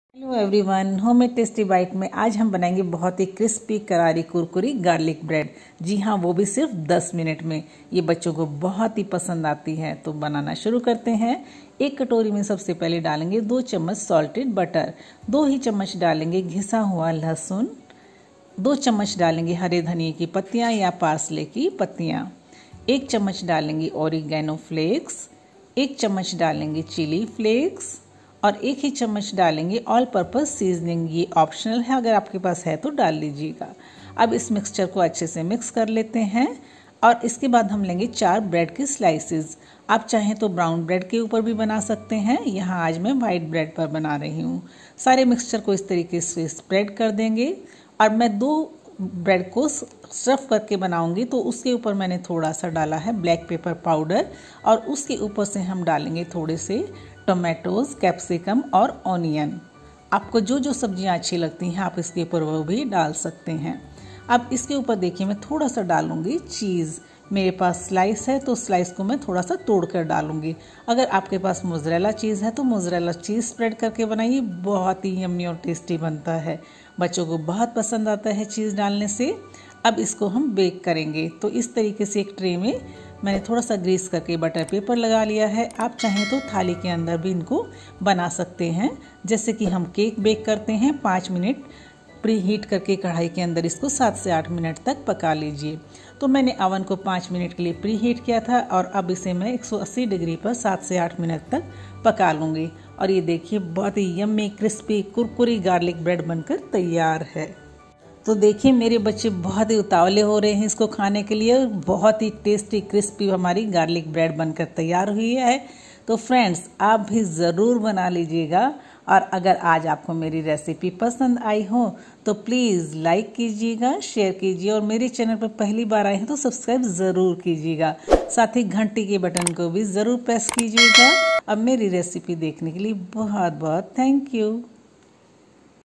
How To Make Perfect Garlic Bread (2 Ways) In Only 10 Minutes!
How To Make Perfect Garlic Bread In Only 10 minutes ( 2 ways)